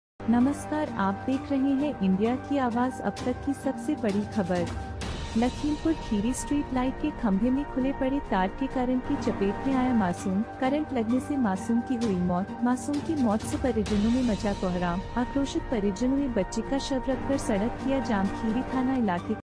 स्ट्रीट लाइट के खंभे में खुले पड़े तार के करंट लगने से मासूम की मौत#indiakiaawaj
स्ट्रीट लाइट के खंभे में खुले पड़े तार के करंट लगने से मासूम की मौत